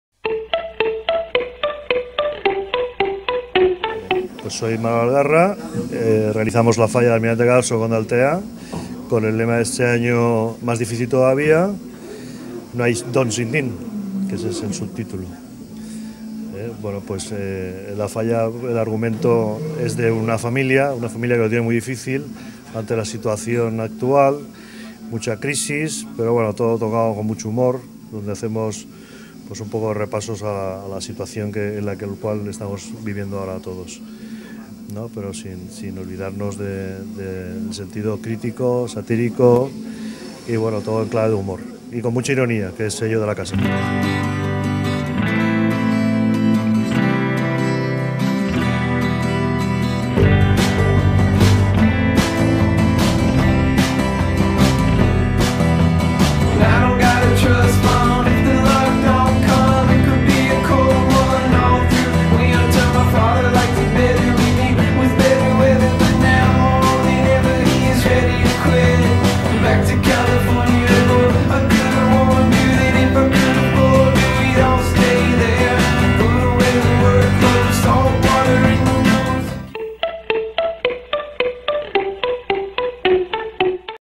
Las Fallas - Manolo Algarra presenta Almirante Cadarso Conde Altea
Presentación del proyecto Almirante Cadarso Conde Altea realizada por su creador Manolo Algarra.También puedes verlo